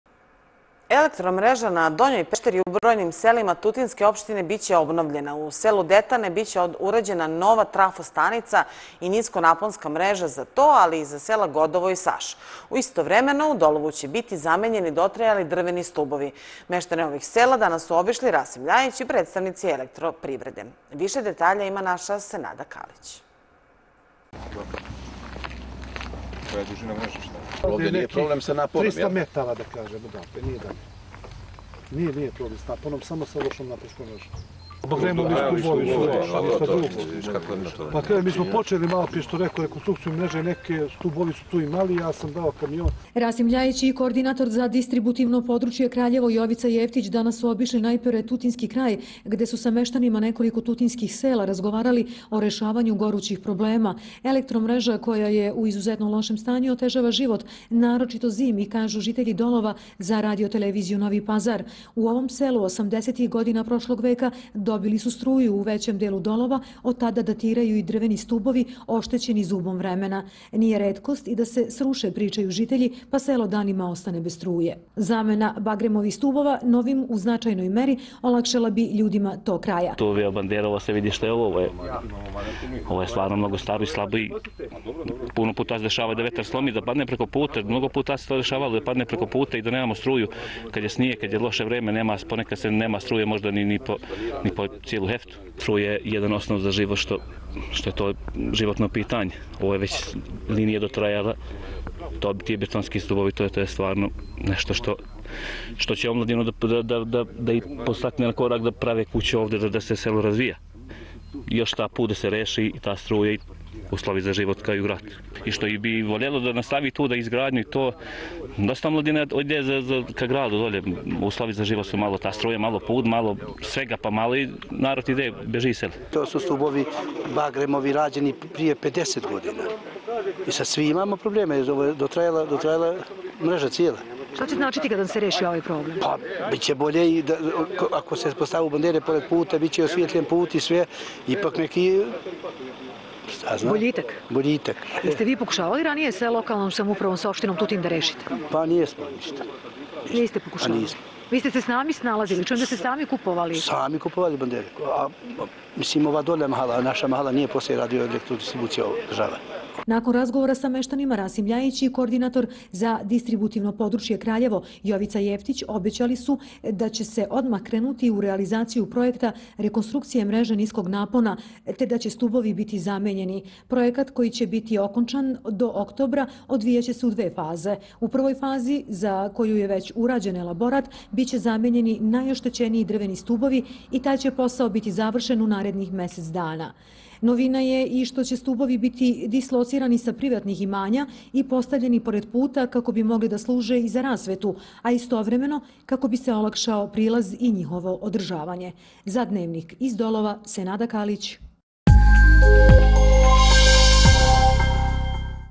Nakon Ljajićeve posete bolje snabdevanje strujom u tutinskim selima
Elektro mreža na donjoj Pešteri, u brojnim selima tutinske opštine biće obnovljena. U selu Detane biće urađena nova trafostanica i niskonaponska mreža za to i sela Godovo i Saš. Istovremeno, u Dolovu će biti zamenjeni dotrajali drveni stubovi. Meštane ovoh sela danas su obišli Rasim Ljajić i predstavnici Elektroprivrede. Više detalja ima naša Senada Kalić.